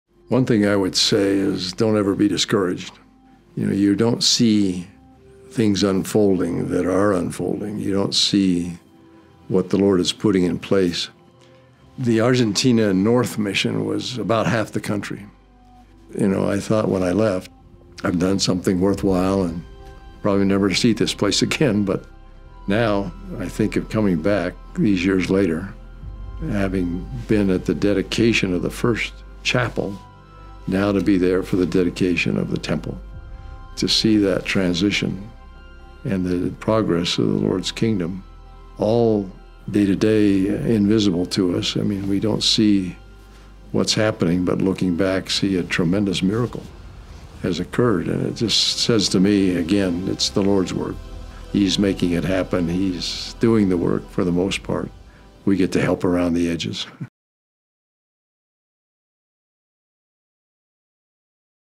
Episode 191 Preview: Elder Christofferson on the Salta Argentina Temple dedication
Elder D. Todd Christofferson of the Quorum of the Twelve Apostles of The Church of Jesus Christ of Latter-day  Saints will dedicate the Salta Argentina Temple on Sunday, June 16, 2024. Having served as a full-time missionary in Argentina, his return is a joyous opportunity to reflect on his own service as well as the growth of the Church in South America. The Salta temple will be the third operating house of the Lord in Argentina and is one of seven dedicated, announced or under construction in the nation. Elder Christofferson joins this episode of the Church News podcast to reflect on his mission and Argentina. He is joined by guest host Church News Managing Editor Scott Taylor, who leads the conversation on service and growth in the nation.